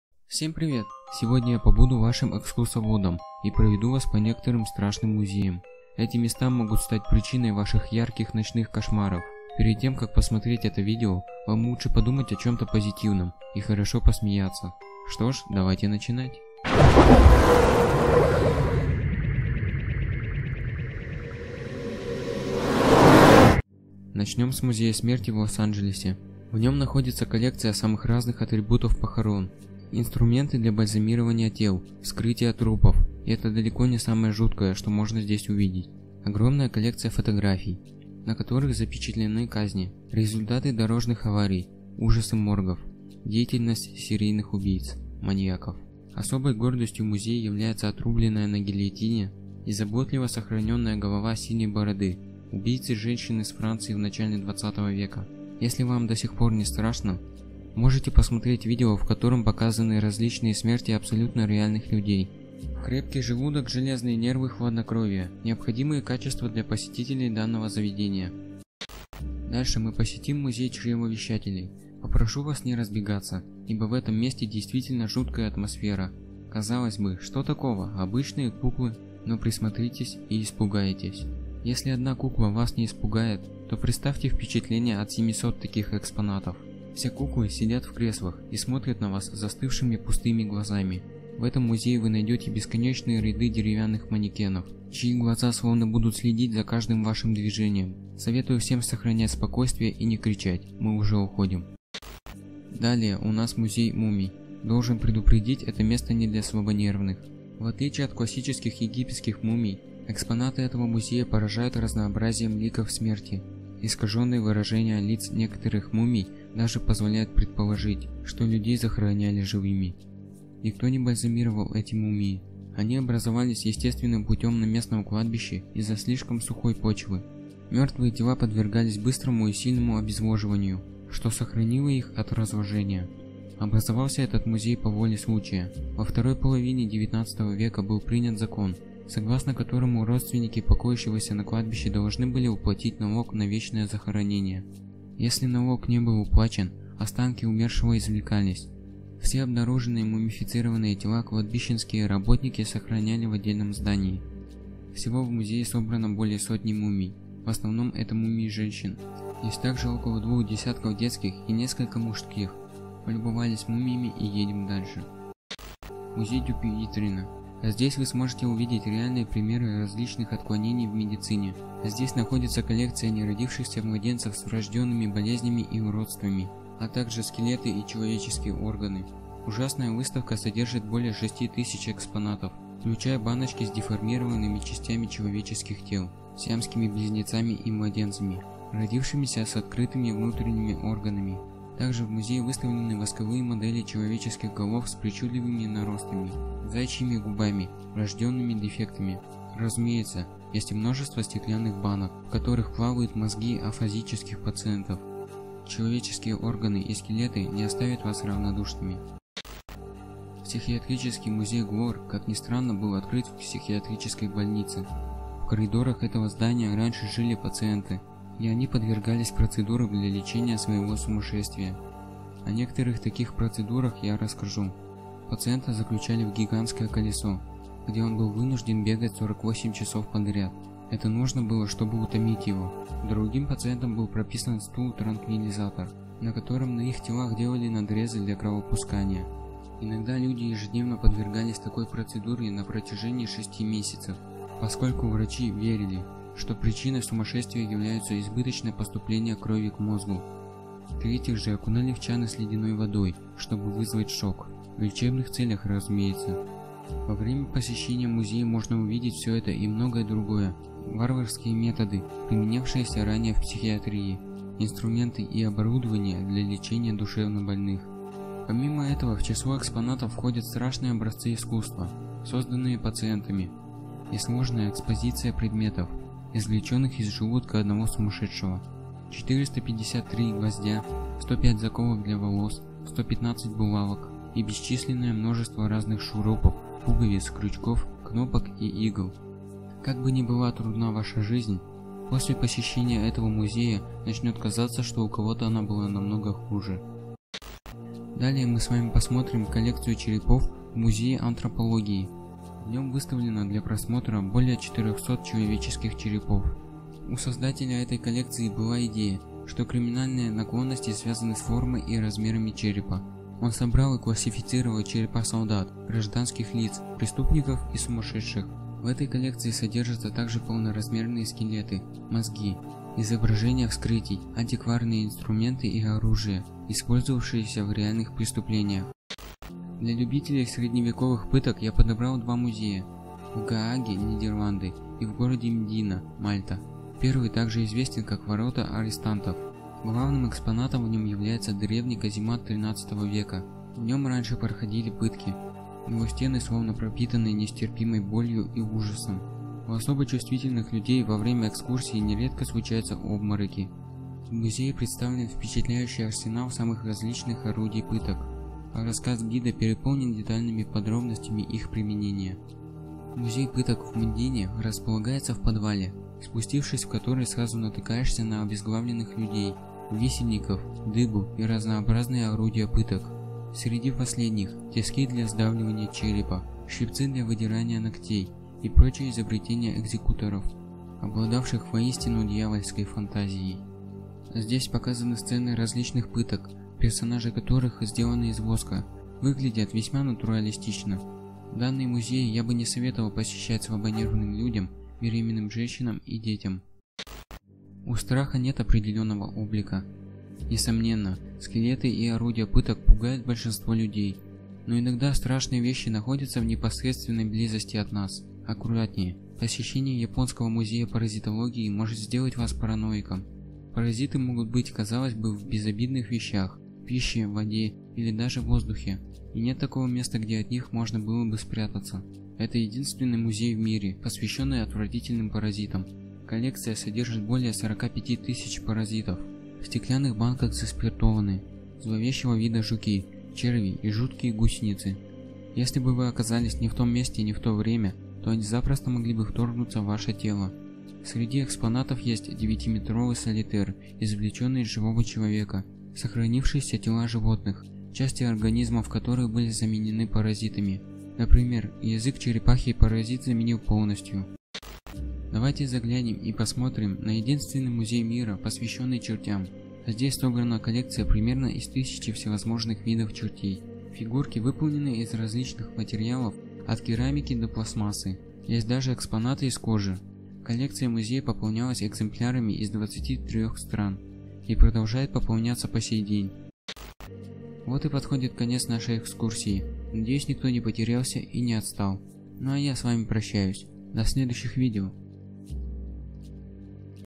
Экскурсия по жутким музеям(+16)
В этом видео я проведу экскурсию по некоторым жутким музеям.